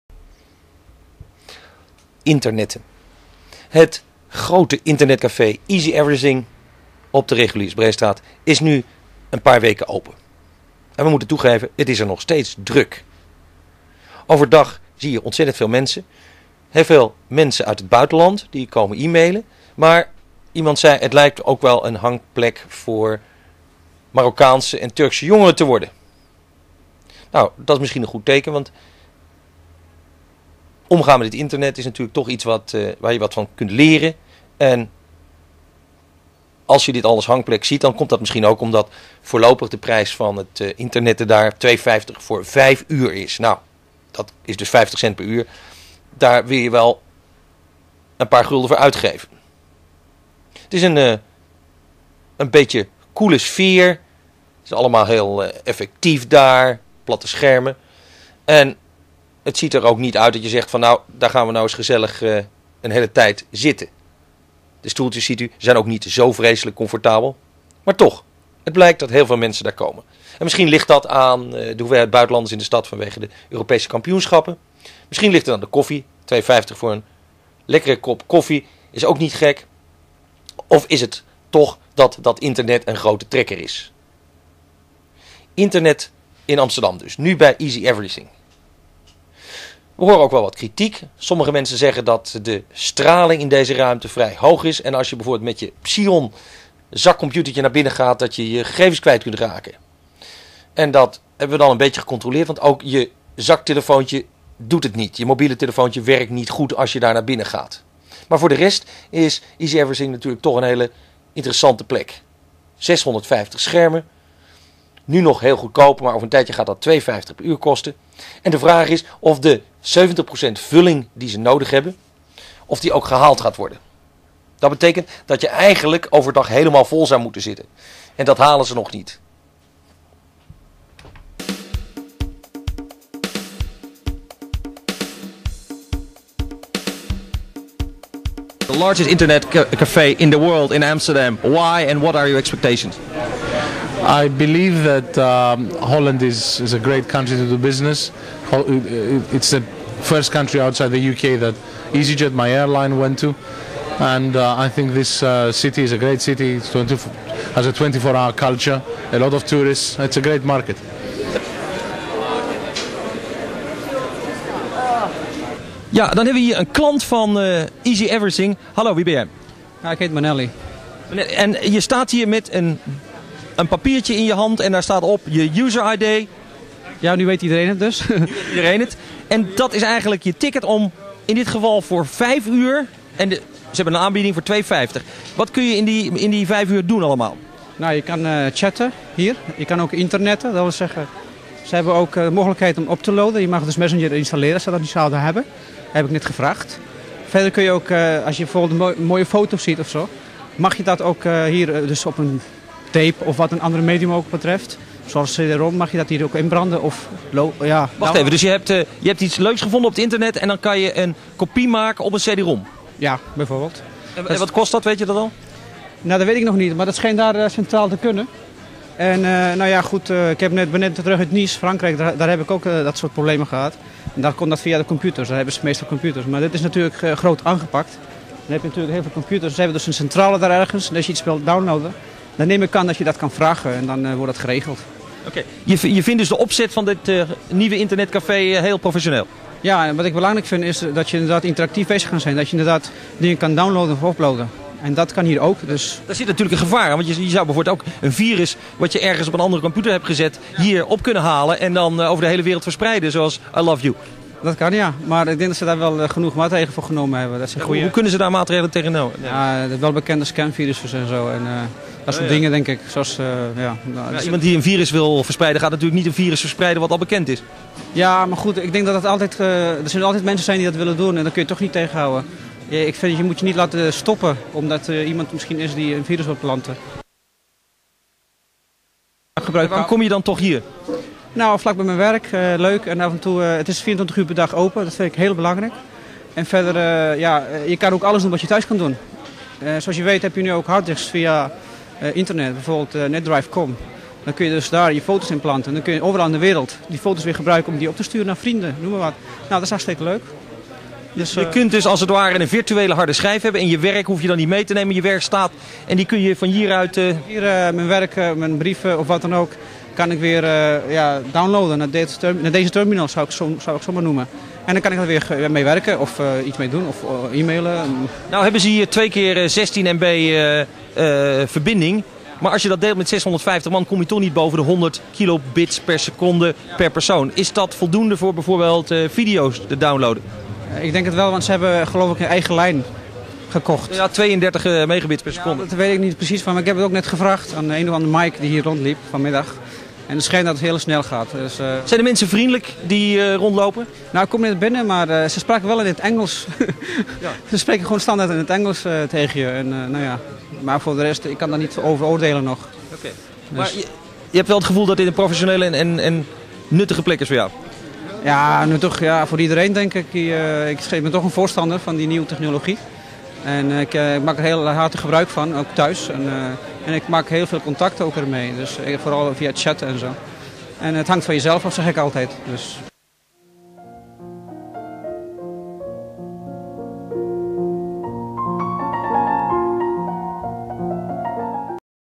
easy-everything internet voor 'n prikkie Stelios Haji-Ioannou 15juni2000
Amsterdamse cultuut en varia, gastronomie, evenementen, feesten, over wat er in Amsterdam gebeurde in de Kleurnet Televisie Amsterdam periode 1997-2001. Kleurnet was een kanaal van Luc Sala en de stichting Kleurnet (Farouk, Ooijens) en zette haar archieven op YouTube. Het internet cafe van Stelios in Amsterdam ging na korte tijd onderuit.